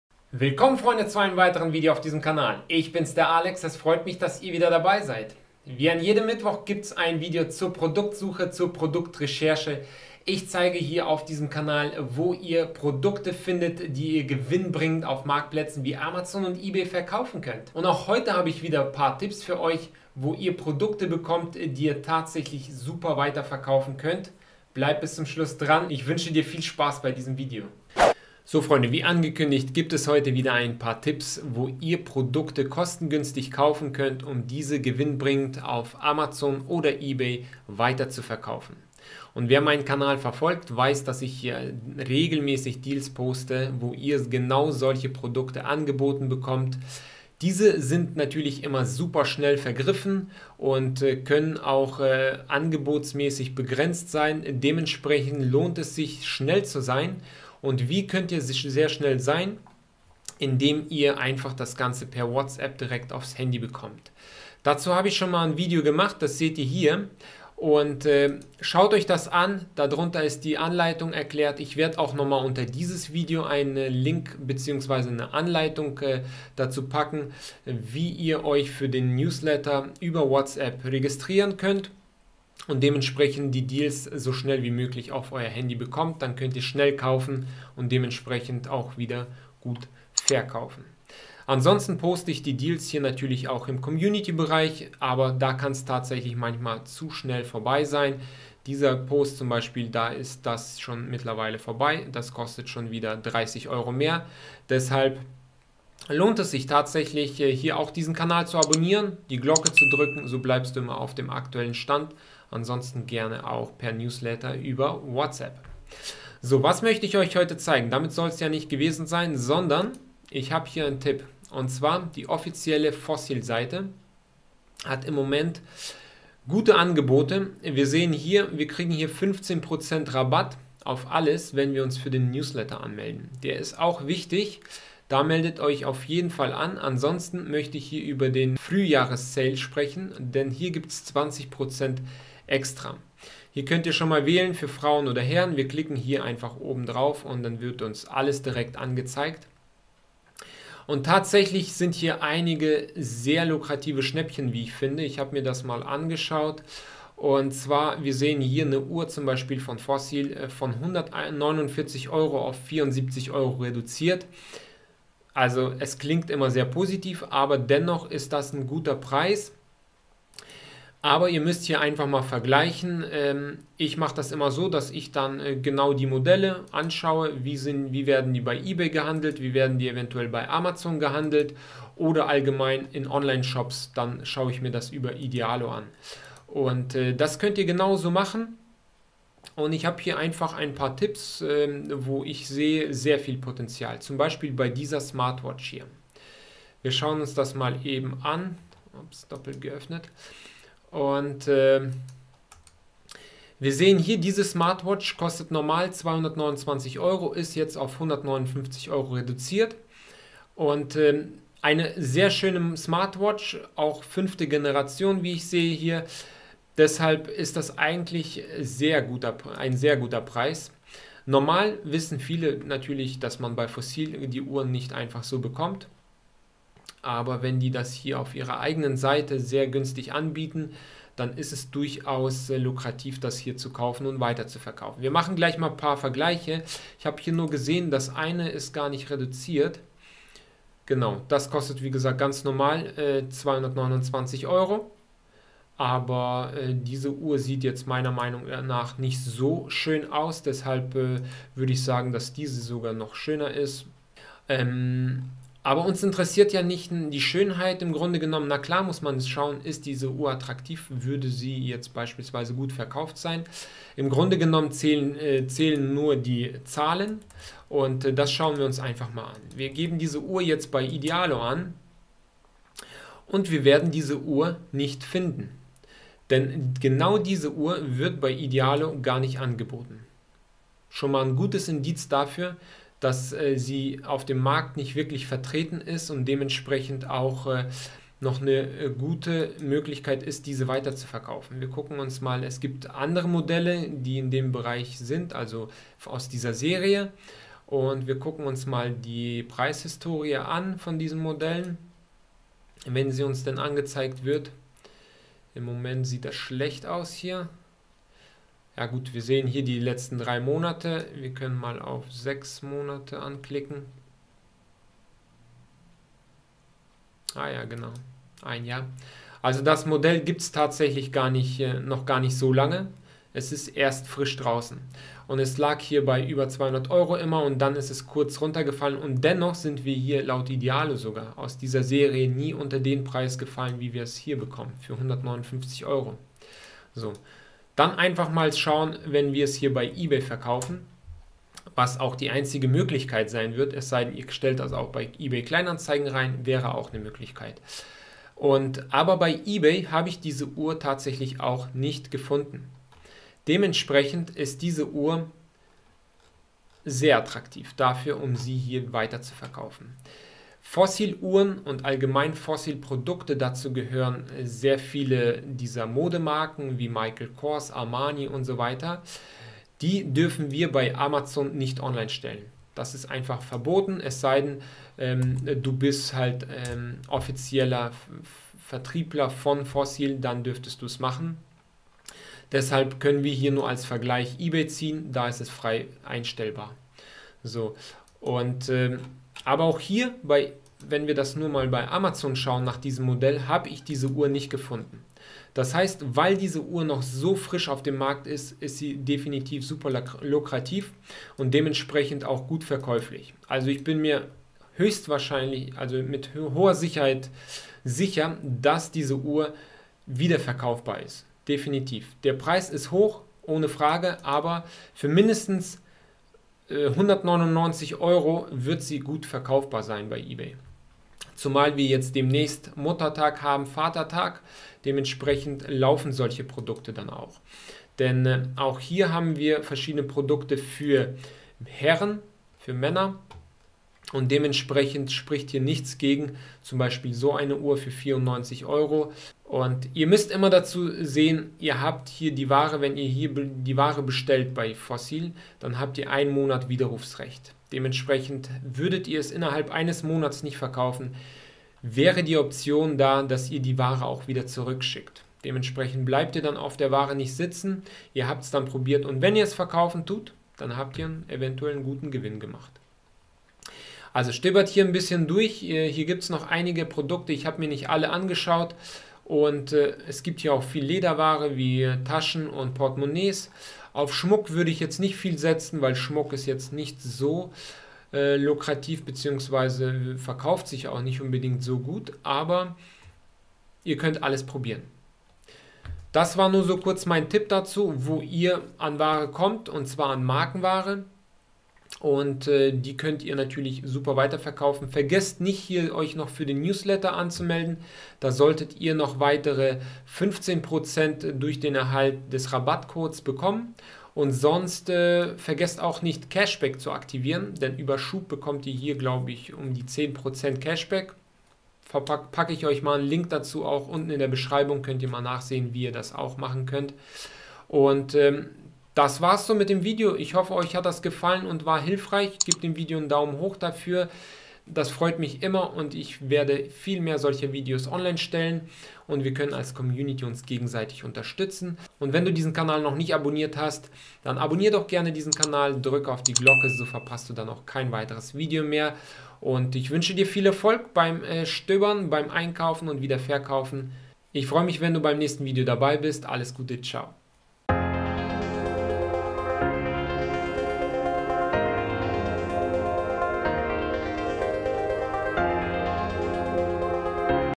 Premium Produkte für Reseller kaufen und verkaufen - Hier bekommst du Schnäppchen
Du bist Reseller und bist ständig auf der Suche nach gewinnbringenden Produkten?

Willkommen im Team, denn auch ich halte immer Ausschau nach den besten Deals und möchte diese mit dir teilen.

In diesem Video zeige ich dir, wo du Schnäppchen für Muttertag bekommst.